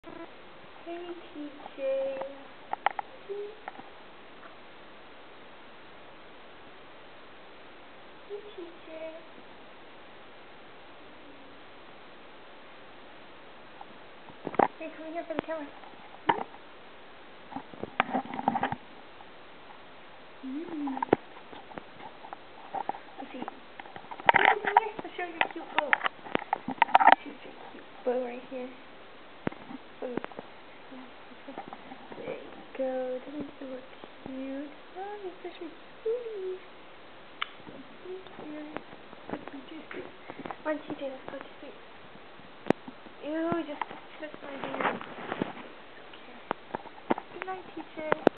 Tj's first video.
July 9 (: his name is Timothy Johnson  [i know silly name] but since the name is WAYYY too long we can it  Tj. for short. His first night at home his such a baby a cute fur ball. :] his also a pomchi